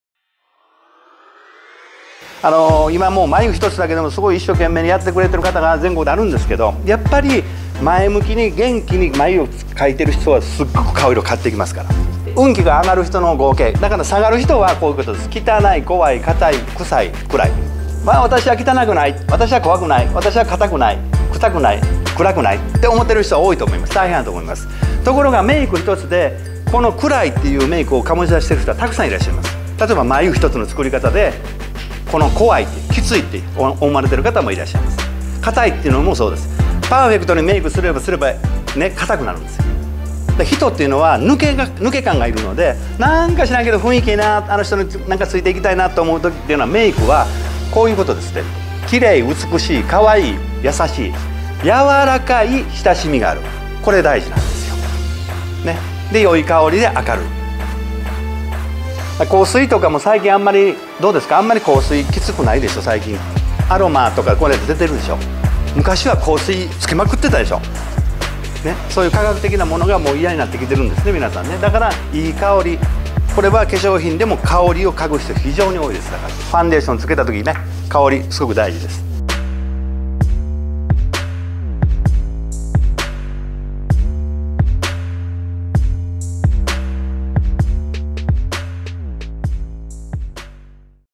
運気のあがるメイクとは（コミュニケーションメイク®講座 富山編）
この動画は、コミュニケーションメイク®講座（カリスマ和美人塾　富山編）の様子。
実技を交えた3時間の講座を行いました。

化粧師秀（けわいしひで）【本名】池端秀之